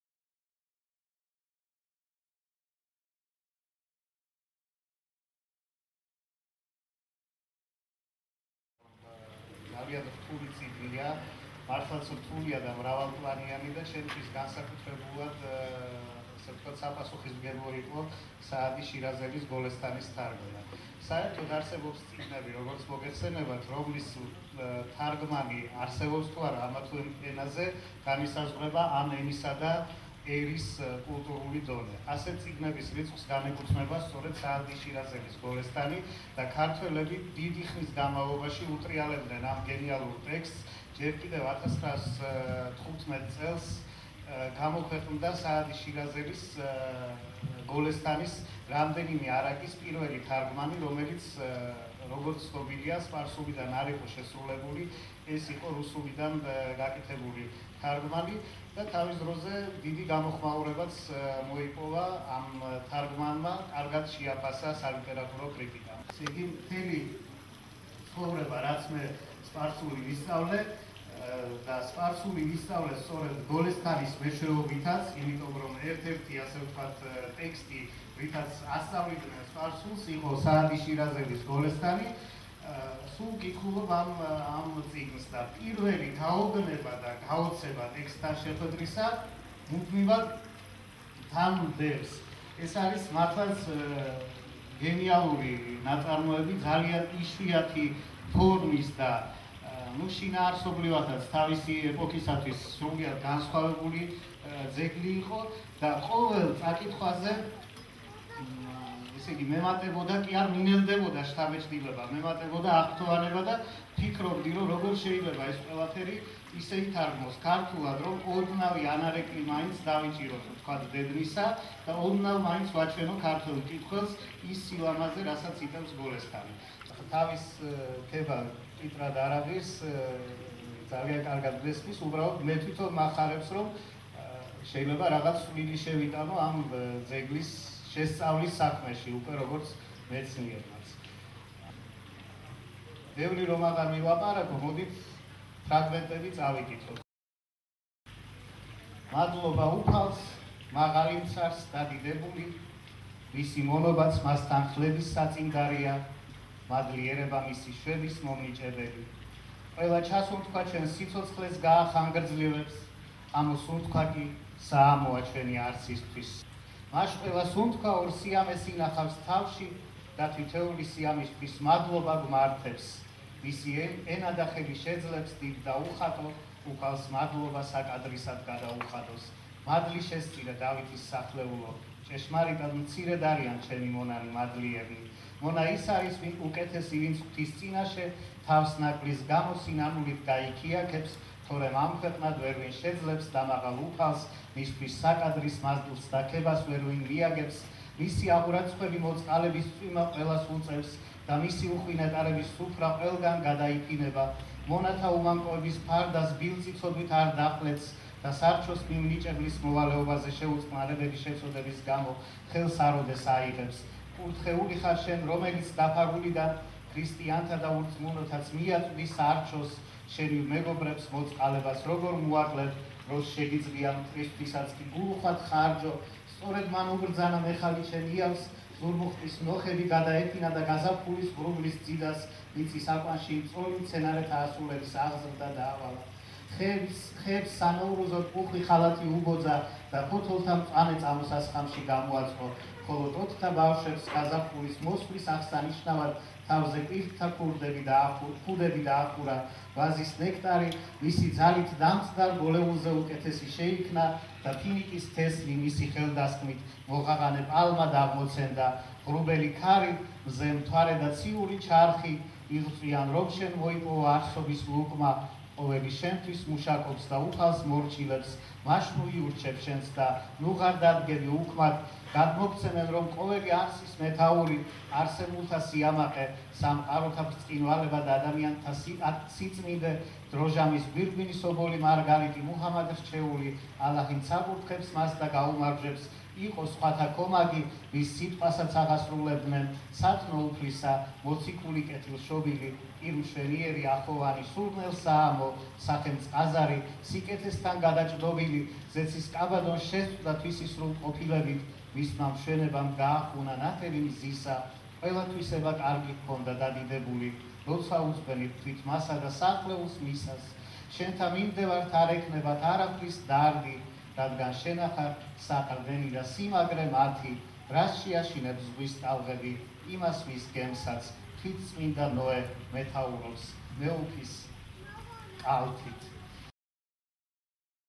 Caucasian House, Presentation of the Book of the Golestan of Sa'di
Giorgi Lobjanidze, famous Georgian orientalist, the author of numerous books and translations form Iranian and Arab languages, representing his new translation of the Golestan Book of Sa'di from Shirazi, the world famous Persian (Iranian) 13th century poet.
Golestan in Persian means "rose-garden" and Golestan of Sa'di is a composition of stories and maxims each representing a flower of this garden. He writes about this garden:
Of what use will be a dish of roses to you
Take one page from this rose garden of mine
A rose only lives for five or six days
The joy from my rose garden always stays

Golestan of Sa'di is world famous due to its eloquence of style, clear language, moral guidance and counseling through the art of poetry. Sa'di himself describes this book as:

"This well-arranged composition will remain for years, When every atom of our dust is dispersed. The intention of this design was that it should survive Because I perceive no stability in my existence, Unless one day a pious man compassionately Utters a prayer for the works of dervishes."